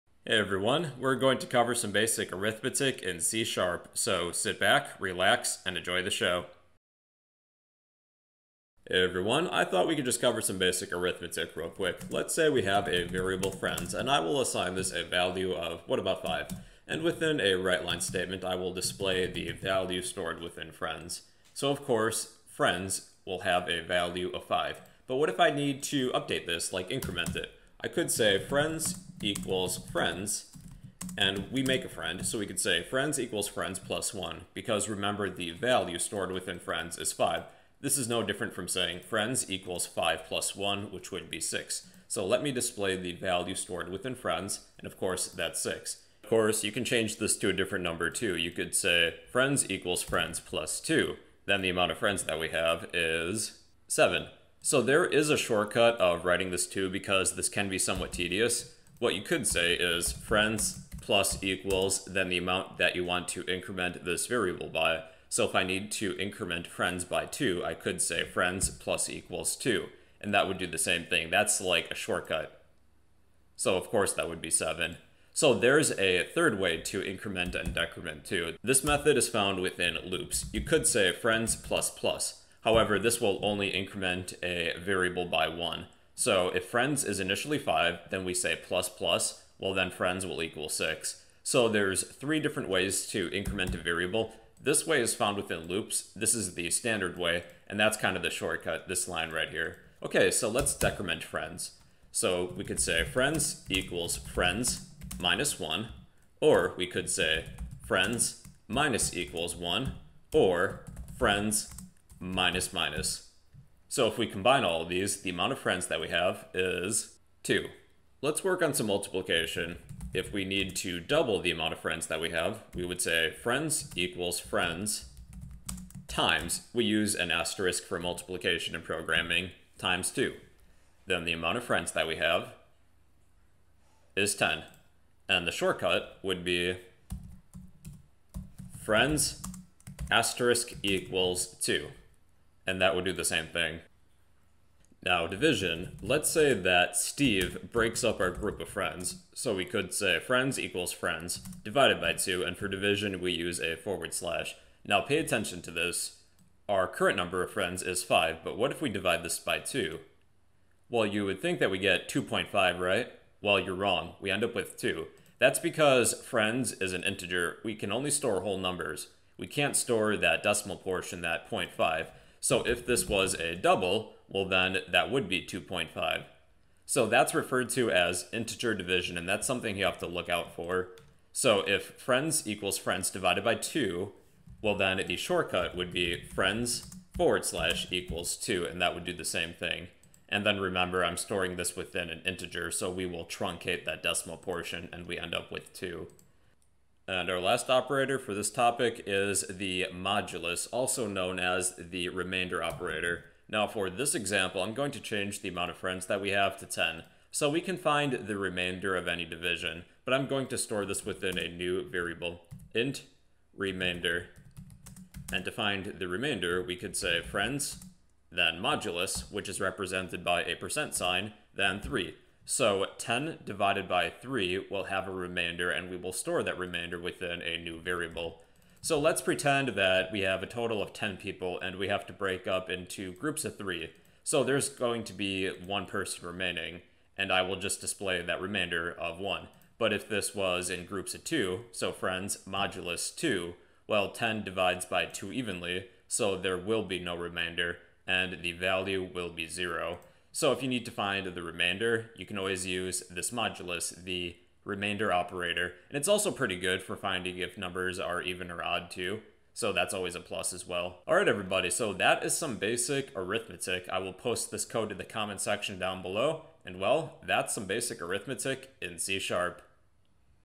C# arithmetic operators 🧮
C# arithmetic operators tutorials example explained

using System;

namespace MyFirstProgram
    class Program
        static void Main(string[] args)
            int friends = 5;

            friends = friends + 1;
            //friends += 1;
            //friends++;

            //friends = friends - 1;
            //friends -= 1;
            //friends--;

            //friends = friends * 2;
            //friends *= 2;

            //friends = friends / 2;
            //friends /= 2;

            //int remainder = friends % 2;
            //Console.WriteLine(remainder);

            Console.WriteLine(friends);